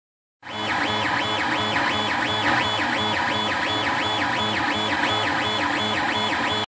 Panoramic Camera Back
Printing of the 6x17 pano camera back